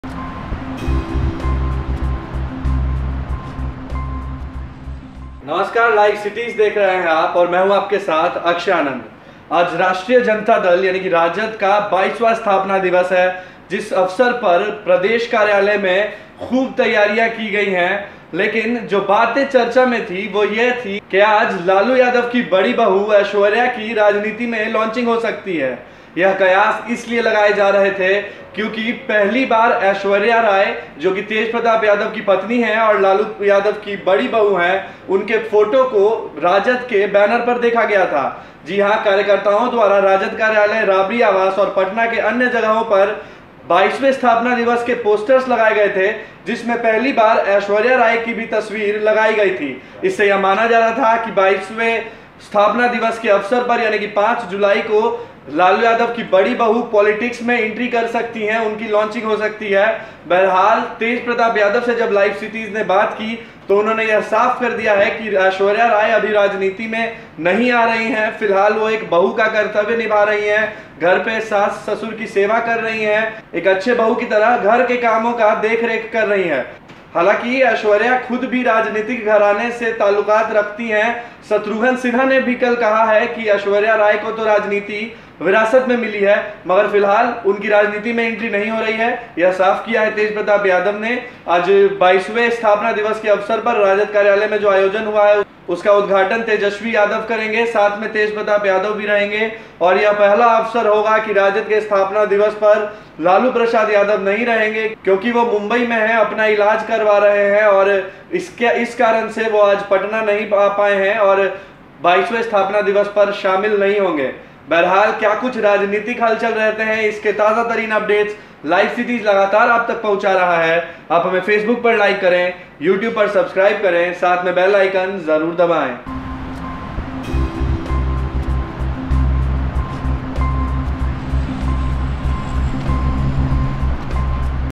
Tej Pratap Yadav बोले- अभी राजनीति में नहीं आएंगी Aishwarya Rai, घर - परिवार की कर रहीं हैं सेवा
Tej Pratap Yadav बोले-  अभी राजनीति में नहीं आएंगी Aishwarya Rai, घर - परिवार की कर रहीं हैं सेवा LiveCities